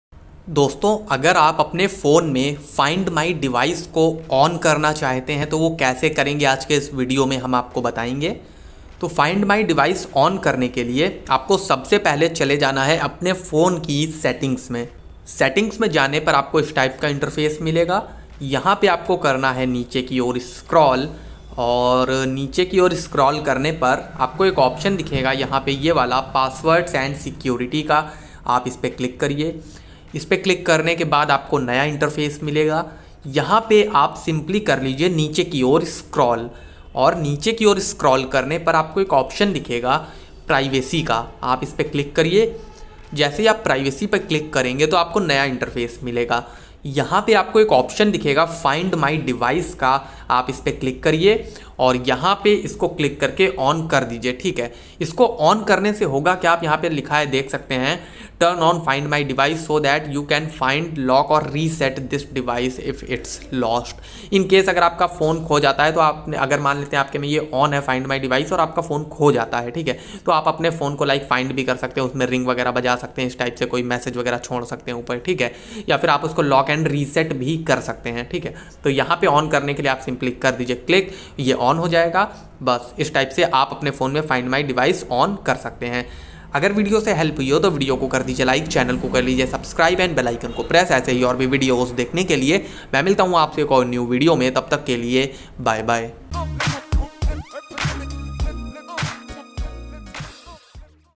Find My Device On Kaise Karen | How To Enable/Turn On Find My Device
1) Find My Device ko on/activate kaise karen
2) how to turn on or enable Find My Device